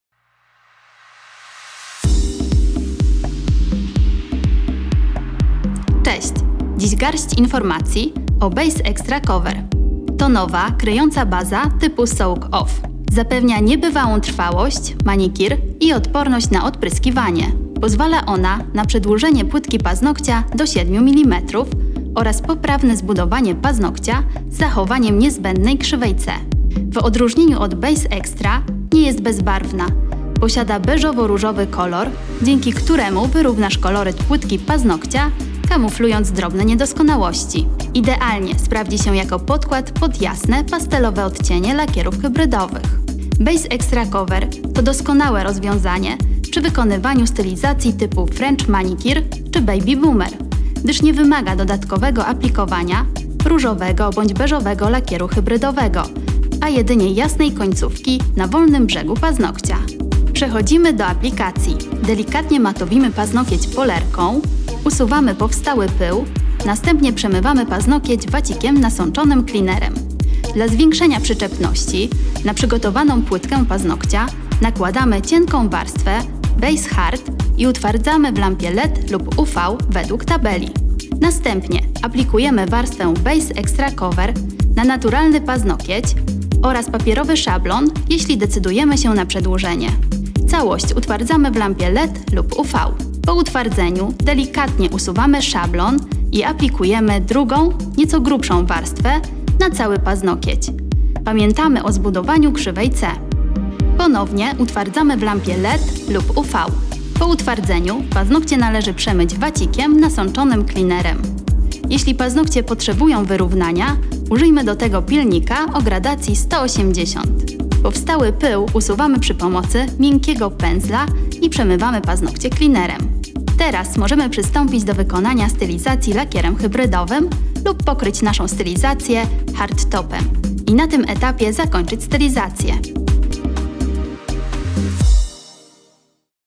Q&A NeoNail - Base Extra Cover
Daj się zachwycić idealną, gęstą konsystencją, doskonałym kryciem i możliwością przedłużenia płytki aż do 7mm! To wszystko możliwe jest dzięki naszej najnowszej bazie - Base Extra Cover ❤ W najnowszym odcinku Q&A usłyszysz najważniejsze informacje o tym jak prawidłowo z niej korzystać. Wypróbujesz ją? 😉